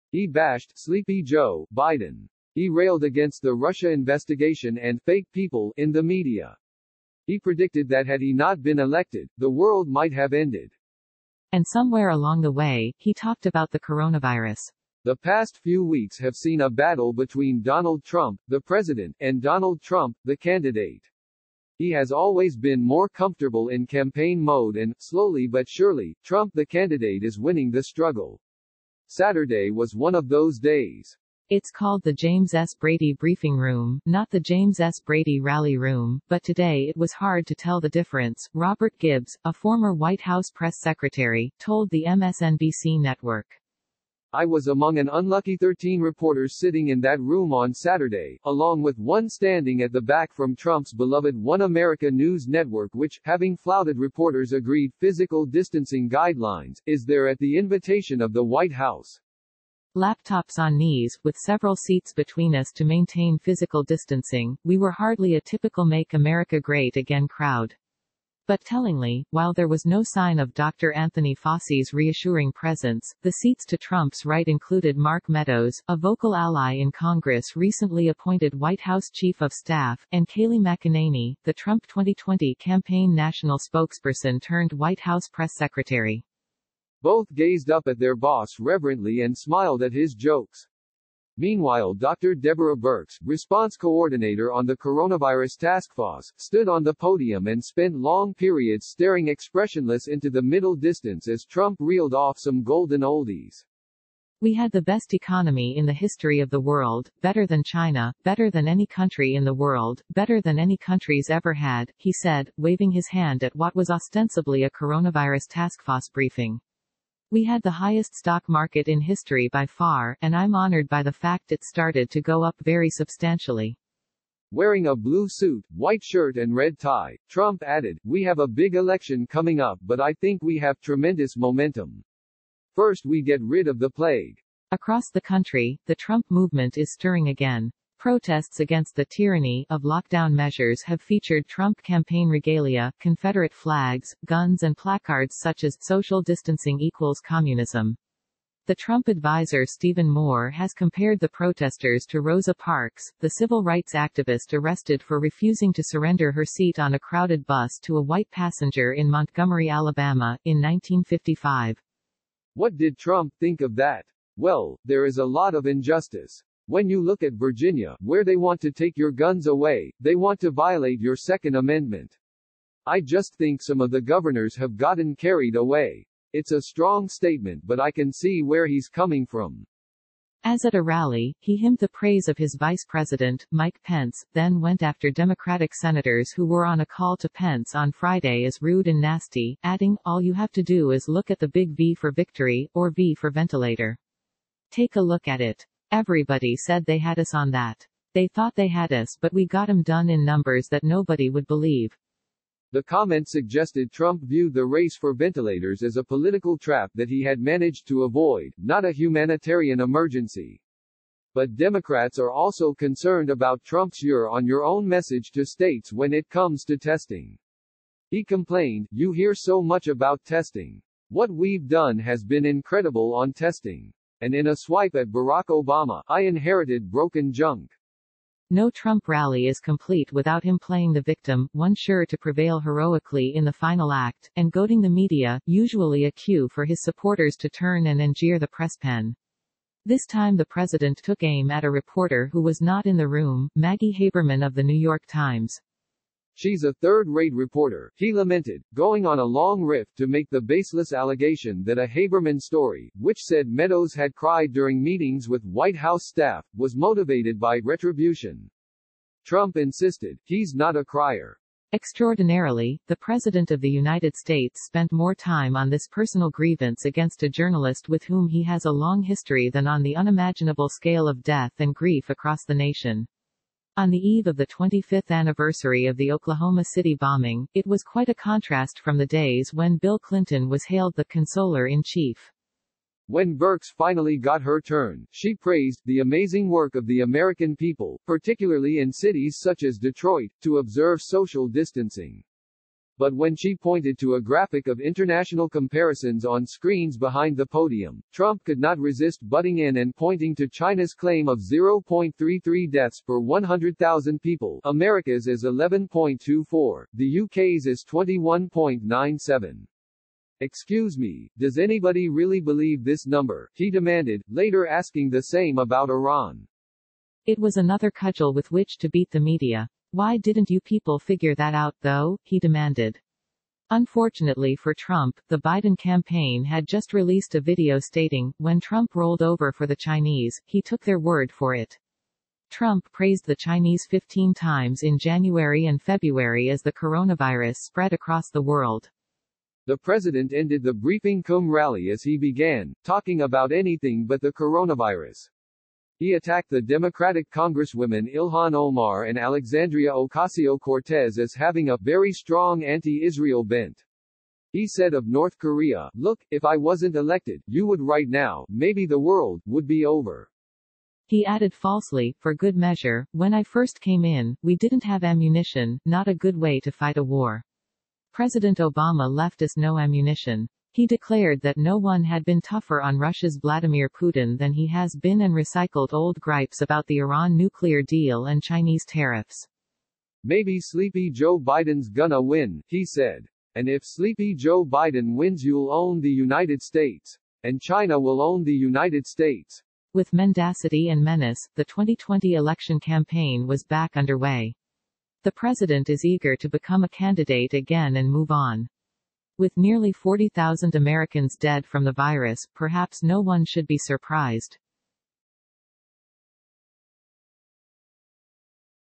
Briefing or rally? Trump shifts to campaign mode as he rails against the media
He bashed “Sleepy Joe” Biden. He railed against the Russia investigation and “fake people” in the media. He predicted that had he not been elected, the world might have ended.